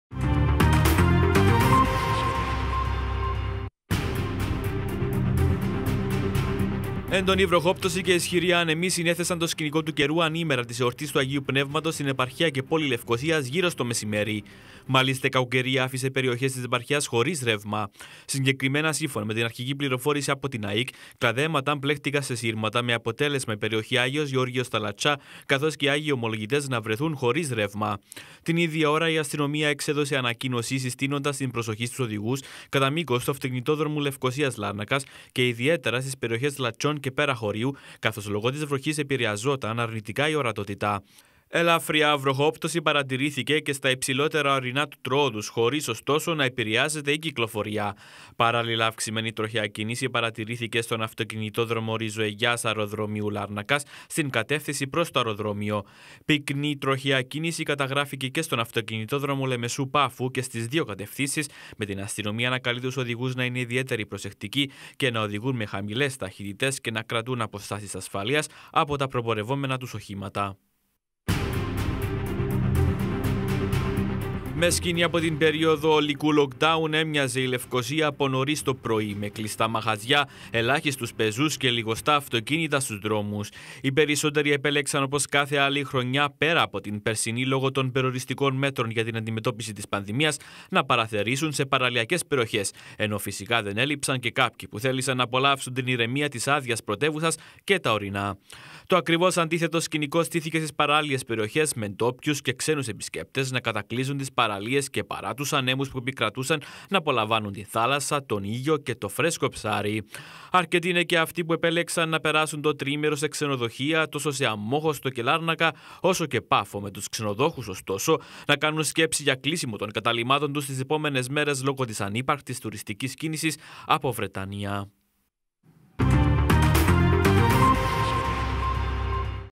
Digital News - ΛΕΥΚΩΣΙΑ: ΒΡΟΧΕΣ ΚΑΙ ΙΣΧΥΡΟΙ ΑΝΕΜΟΙ - ΠΕΡΙΟΧΕΣ ΧΩΡΙΣ ΡΕΥΜΑ - [21/06/21]
Άδειοι οι δρόμοι της πρωτεύουσας ανήμερα της εορτής του Αγίου Πνεύματος, ενώ το σκηνικό του καιρού το μεσημέρι στην επαρχία Λευκωσίας θύμισε κατακλυσμό, με περιοχές να μένουν χωρίς ρεύμα. Πολύ διαφορετικό το σκηνικό στις παράλιες περιοχές.